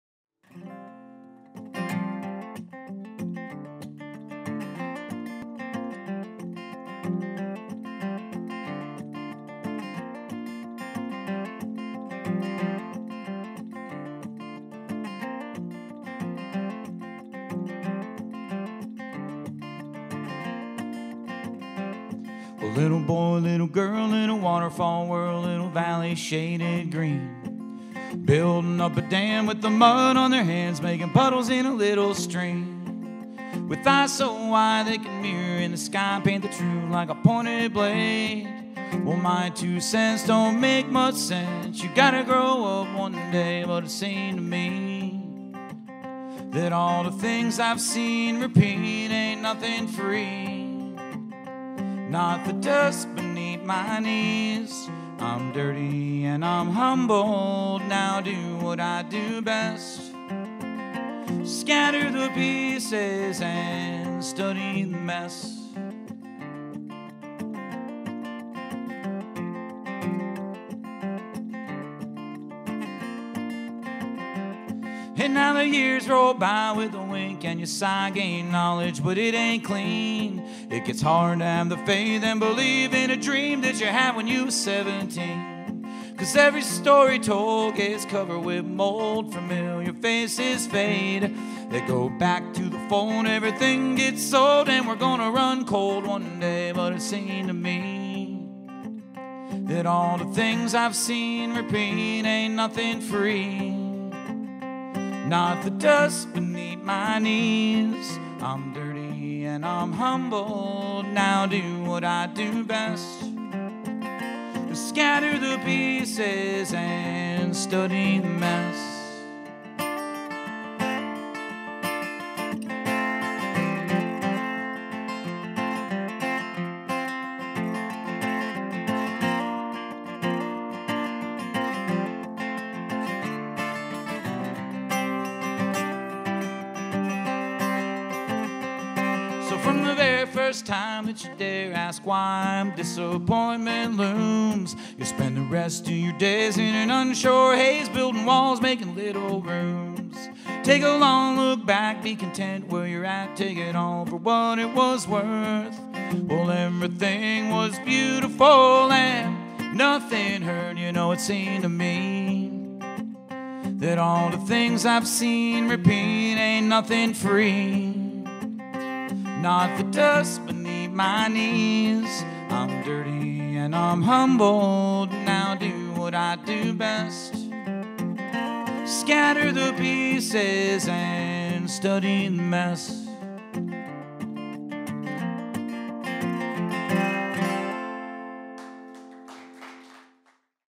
Gary Antol Song 2
Gary Antol's performance on November 3rd at Rhythm House Studios.